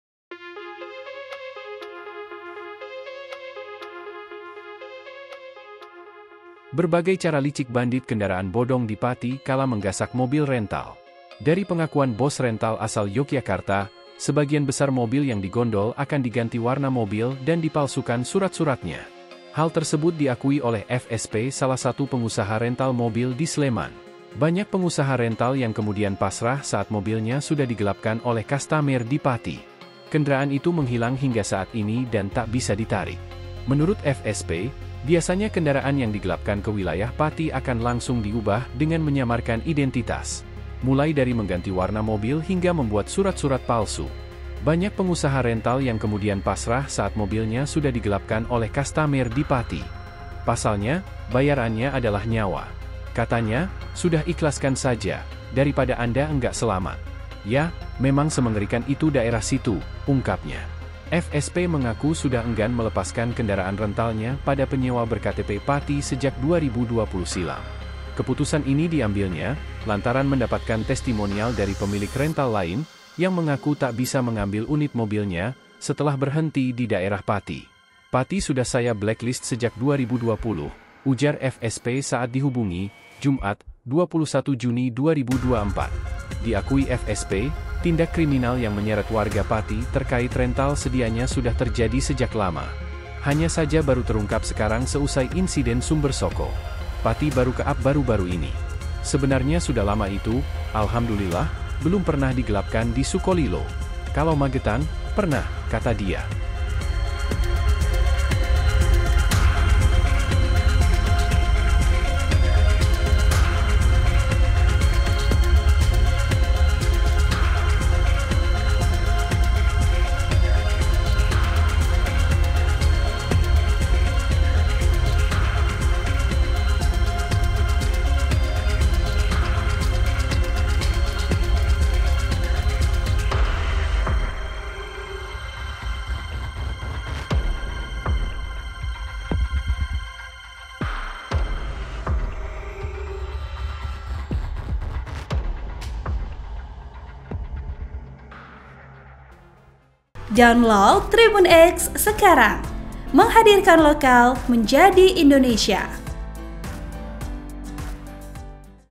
Siasat Licik Penadah Asal Pati Curi Mobil Bos Rental, Dicat Ulang hingga Bikin Surat-surat Palsu
SURYA.CO.ID - Berbagai cara licik bandit kendaraan bodong di Pati kala menggasak mobil rental.

Dari pengakuan bos rental asal Yogyakarta, sebagian besar mobil yang digondol akan diganti cat dan dipalsukan surat-suratnya.

Hal ini diakui oleh FSP salah satu pengusaha rental mobil di Sleman.

FSP bercerita bahwa empat tahun silam, rekannya asal Jawa Barat pernah meminjamkan mobil rental untuk kustomer asal Pati.

Kendraan itu menghilang hingga saat ini dan tak bisa ditarik.

Menurut FSP, biasanya kendaraan yang digelapkan ke wilayah Pati akan langsung diubah dengan menyamarkan identitas.

Mulai dari mengganti warna cat hingga membuat surat-surat palsu.

"Bukan hilang, (unitnya) enggak bisa diambil sampai sekarang. Entah ganti cat, entah dibikinin surat bodong, pokoknya unit itu ya enggak bisa diambil. GPS-nya itu enggak dicabut, berhenti di lokasi itu," ujar FSP.

Banyak pengusaha rental yang kemudian pasrah saat mobilnya sudah digelapkan oleh kustomer di Pati.

Pasalnya, bayarannya adalah nyawa.

"(Katanya), sudah ikhlaskan saja, daripada Anda enggak selamat. Ya, memang semengerikan itu daerah situ," ungkapnya.

FSP mengaku sudah enggan melepaskan kendaraan rentalnya pada penyewa ber-KTP pati sejak 2020 silam.

Keputusan ini diambilnya, lantaran mendapatkan testimonial dari pemilik rental lain, yang mengaku tak bisa mengambil unit mobilnya, setelah berhenti di daerah Pati.

"(Pati) sudah saya blacklist sejak 2020. Jadi, dulu, awal-awal Covid itu teman rental ada yang menginformasikan di grup WhatsApp kalau mobilnya ke arah sana dan enggak bisa diambil," ujar FSP saat dihubungi, Jumat (21/6/2024).

Diakui FSP, tindak kriminal yang menyeret warga Pati terkait rental sedianya sudah terjadi sejak lama.

Hanya saja baru terungkap sekarang seusai insiden Sumbersoko.

"Pati baru ke up baru-baru ini, sebenarnya sudah lama itu."

"Alhamdulillah, belum pernah digelapkan di Sukolilo. Kalau Magetan, pernah," kata dia.